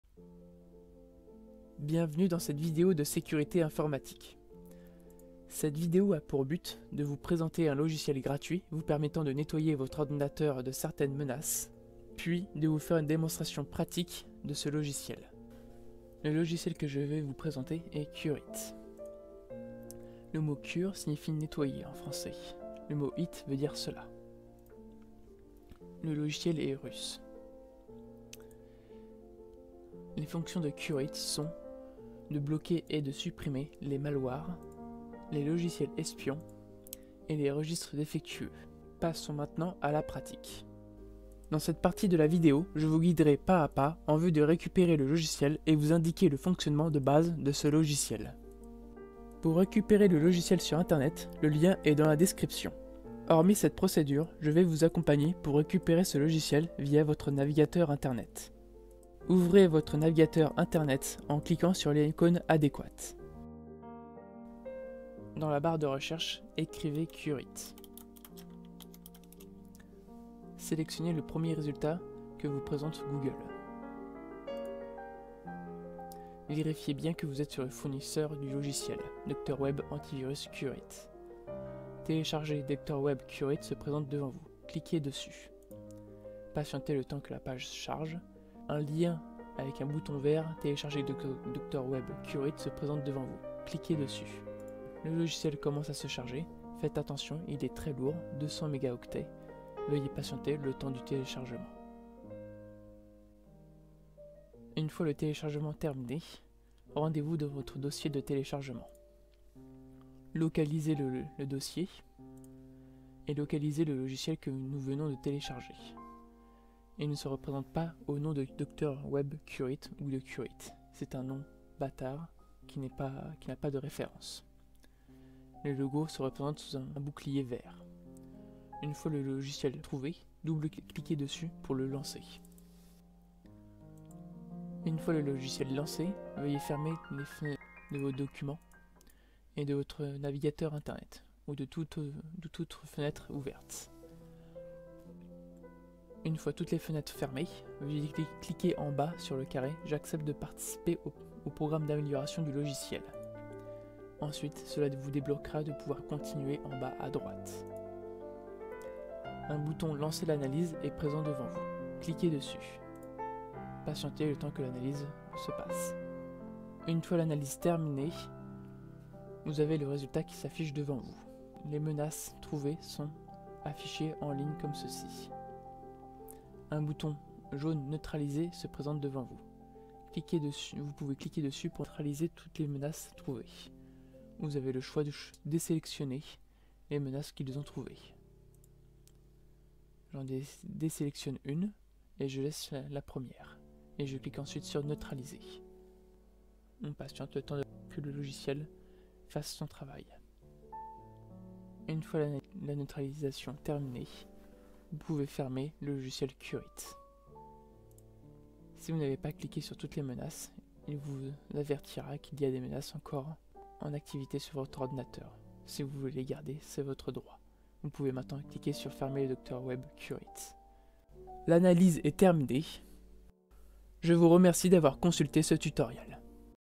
Sécurité : Dr.Web CureIt
Frank vous présente aujourd'hui le logiciel Dr.Web CureIT! qui permet d’examiner votre ordinateur pour détecter d’éventuelles infections par des virus ou des spywares.